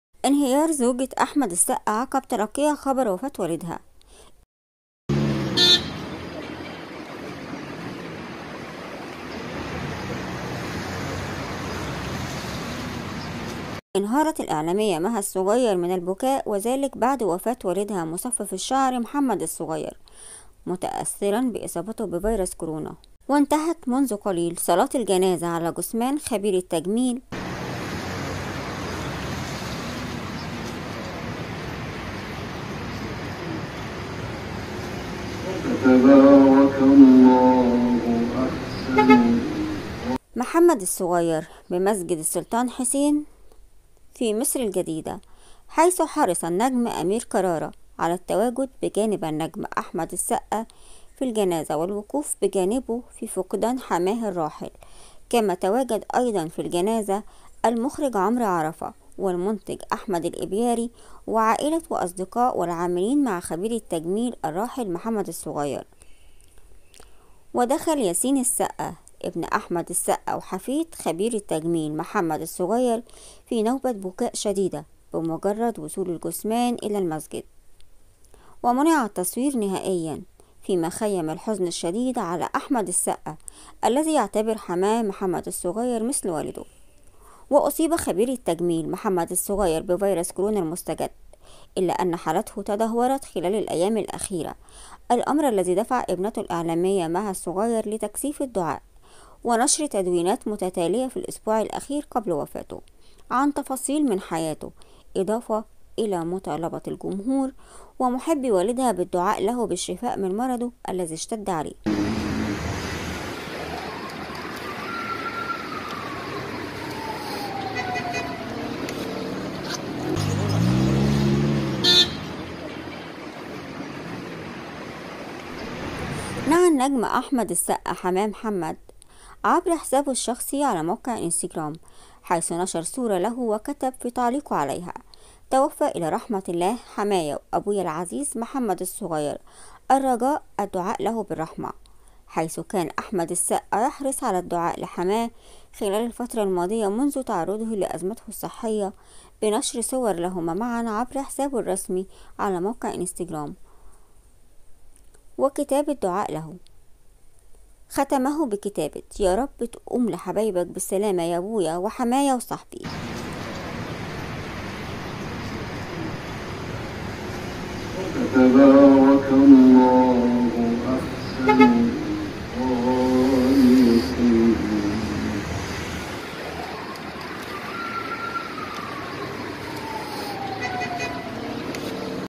انهيار زوجة احمد السقا بعد وفاة والدها واول لقطات من الجنازه
انهارت مهاالصغير فور سمعها خبر وفاة والدها محمد الصغير مصفف الشعر وخبير التجميل.. وشيعت جنازة الراحل محمدالصغير بعد صلاة الظهر وحرص الفنان#اميركراره علي الوقوف بجانب#احمدالسقا في تشيع جنازة حماه